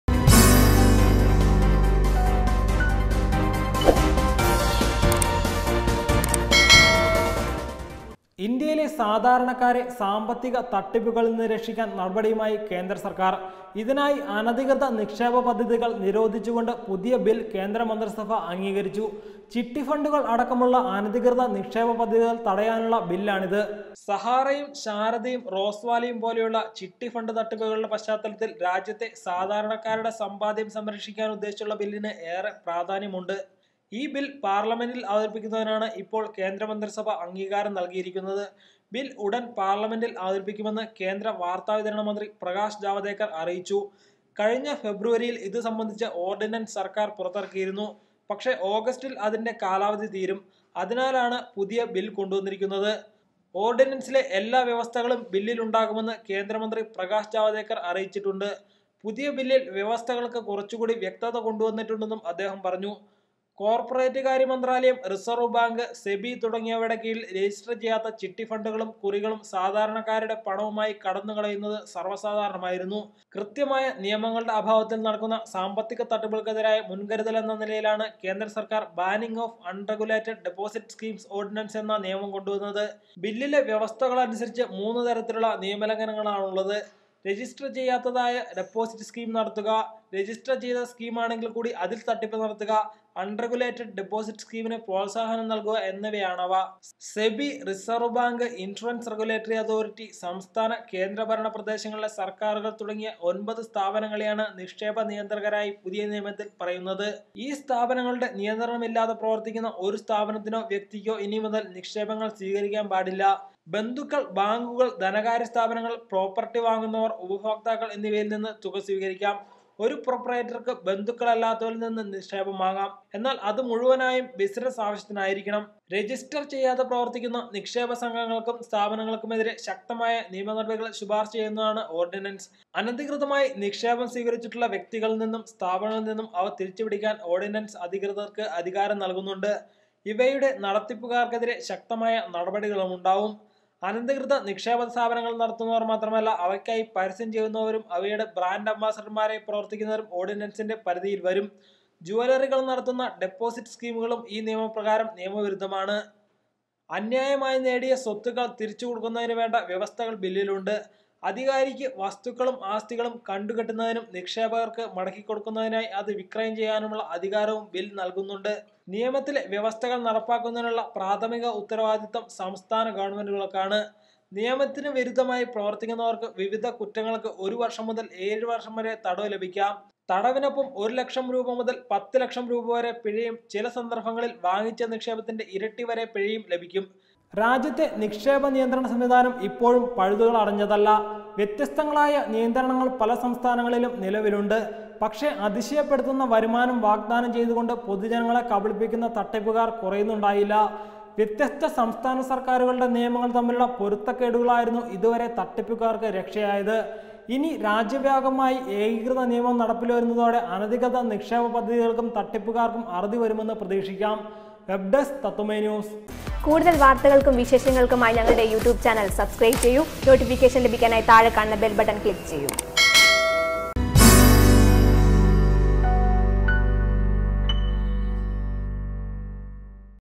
സാധാരണക്കാരെ സാമ്പത്തിക തട്ടിപ്പുകളിൽ നിന്ന് രക്ഷിക്കാൻ നിയമവുമായി കേന്ദ്രസർക്കാർ
ഇന്ത്യയിലെ സാധാരണക്കാരെ സാമ്പത്തിക തട്ടിപ്പുകളിൽ നിന്ന് രക്ഷിക്കാൻ നടപടിയുമായി കേന്ദ്രസർക്കാർ. ഇതിനായി   അനധികൃത നിക്ഷേപ പദ്ധതികൾ നിരോധിച്ചുകൊണ്ട് പുതിയ ബിൽ കേന്ദ്ര മന്ത്രിസഭ അംഗീകരിച്ചു. ചി​ട്ടി​ഫ​ണ്ടു​ക​ൾ അ​ട​ക്ക​മു​ള്ള അ​ന​ധി​കൃ​ത നി​ക്ഷേ​പ പ​ദ്ധ​തി​ക​ൾ ത​ട​യാ​നു​ള്ള ബി​ൽ ആണിത് . സഹാറയും ശാരദയും റോസ് വാലിയും  പോലെയുള്ള  ചിട്ടി ഫണ്ട് തട്ടിപ്പുകളുടെ പശ്ചാത്തലത്തിൽ , രാജ്യത്തെ സാധാരണക്കാരുടെ സമ്പാദ്യം സംരക്ഷിക്കാൻ ഉദ്ദേശിച്ചുള്ള ബില്ലിന് ഏറെ പ്രാധാന്യമുണ്ട്.